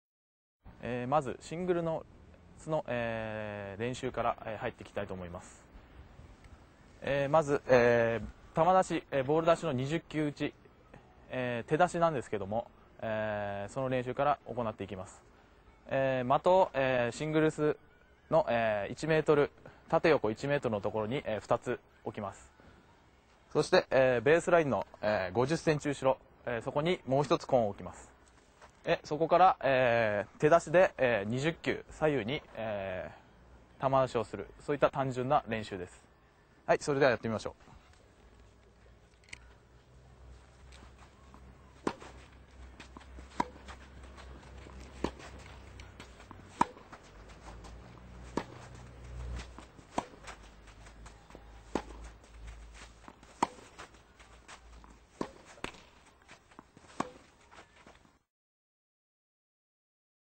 沖縄尚学 総合力で勝つレベルアップトレーニング
ジャパンライムDVD 【硬式テニス】
『沖縄尚学 総合力で勝つレベルアップトレーニング～部活指導の可能性に迫る～』

■指導・解説：平良　和己（沖縄尚学高等学校テニス部監督）
■実技協力：沖縄県／沖縄尚学高等学校テニス部